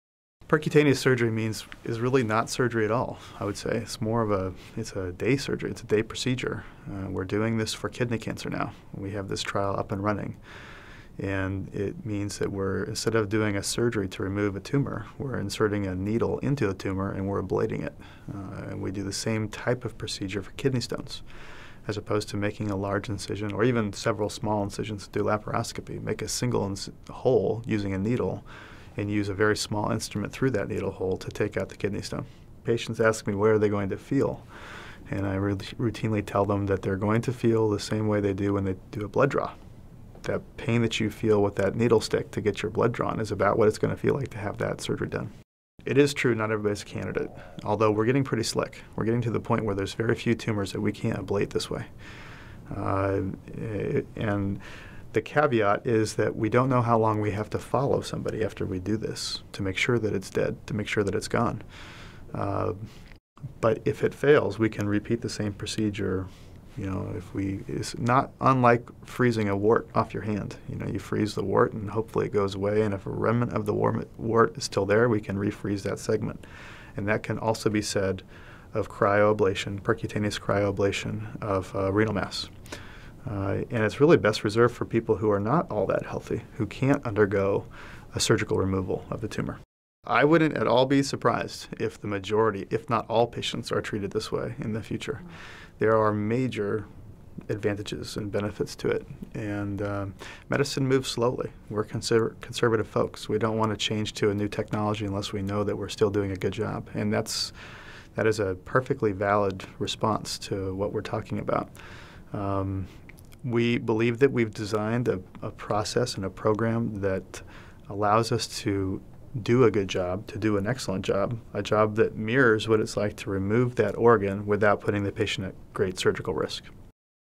Dr. Douglas Sutherland Talks Percutaneous Surgery
Douglas Sutherland, MD, Chief of Urology, discusses percutaneous surgery and how it applies to kidney cancer.